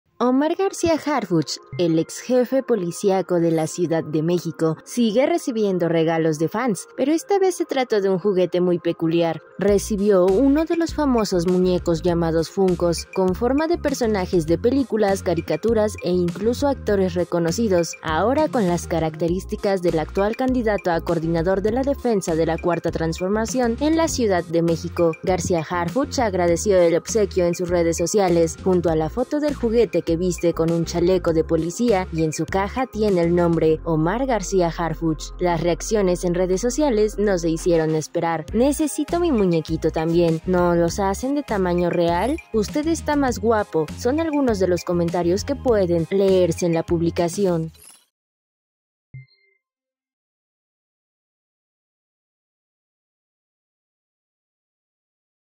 Crean funko de Omar García Harfuch; así reaccionó
Las fans del aspirante a coordinar la 4T en CDMX, aprovecharon para lanzarle piropos y cumplidos